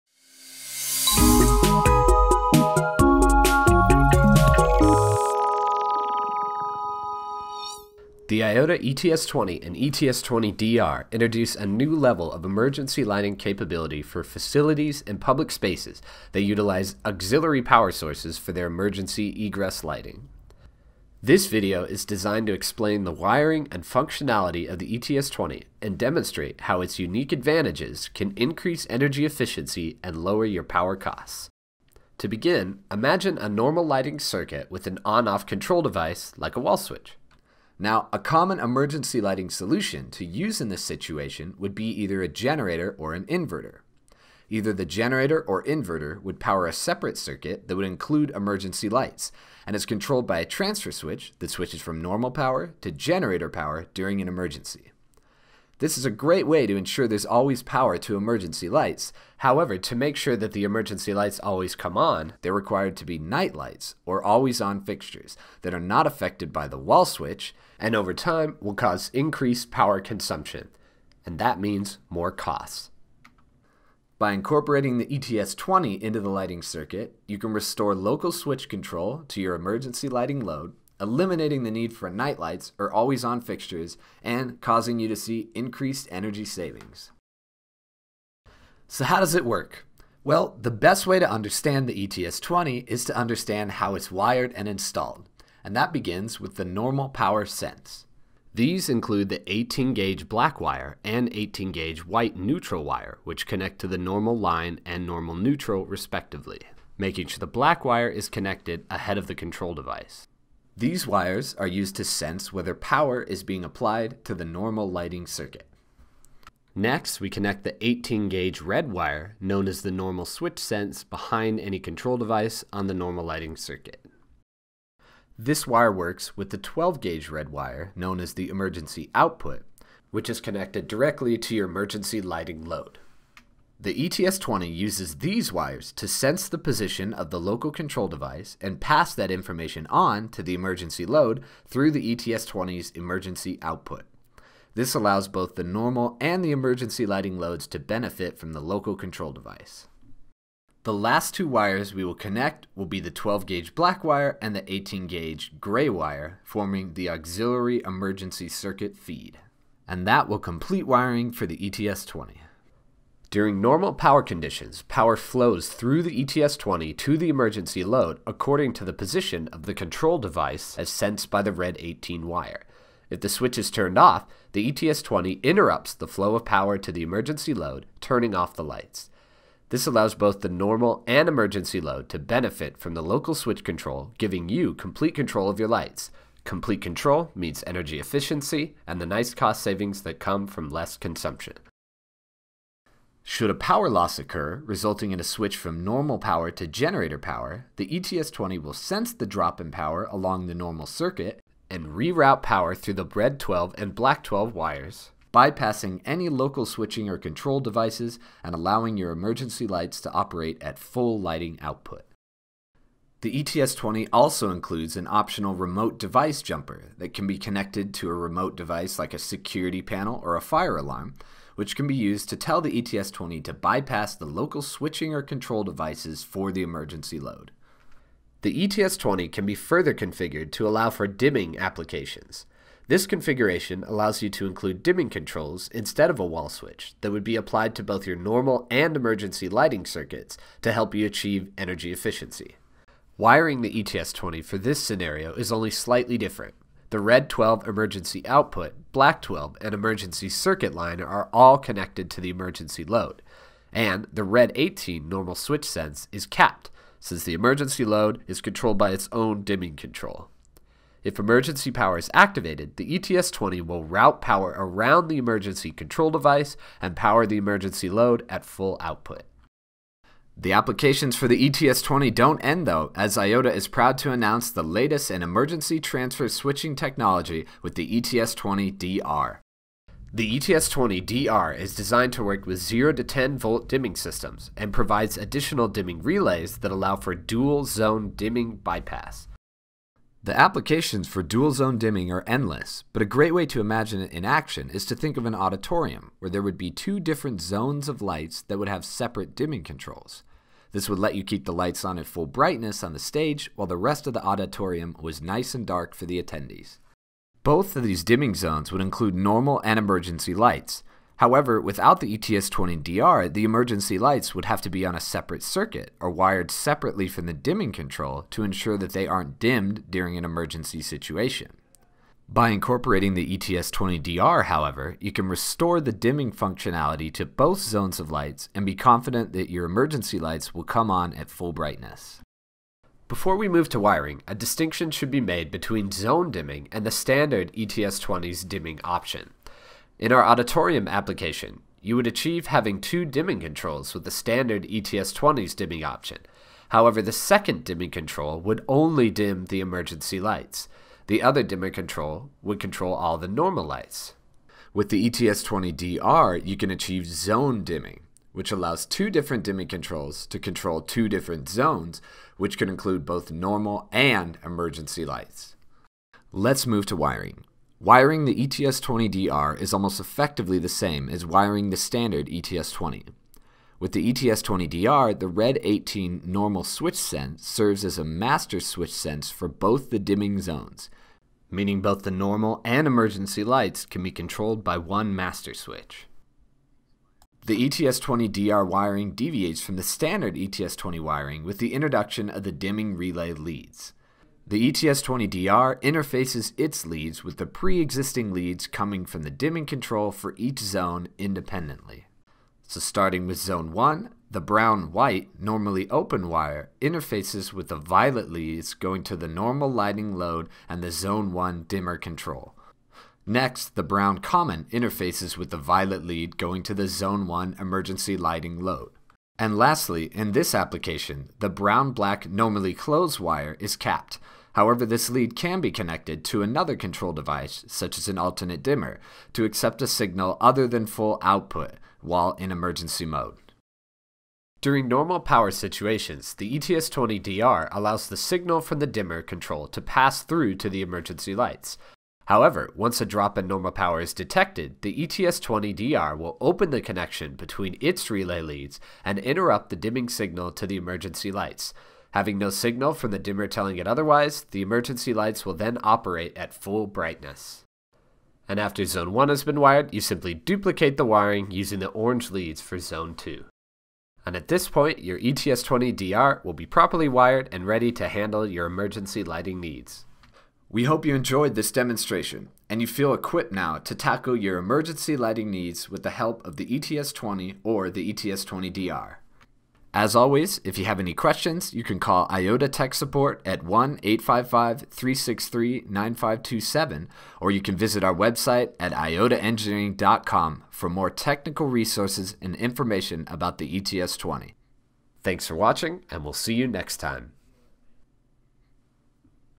Enhanced Emergency Lighting Control and Energy Savings with the IOTA ETS-20 and ETS-20-DR
The IOTA ETS-20 introduces a new level of emergency lighting capability to facilities and public spaces that use auxiliary power sources for their emergency lighting.  This video demonstrates the unique features of the ETS-20, breaks down the wiring and installation, and shows how the ETS-20 can increase your energy performance and reduce your power consumption.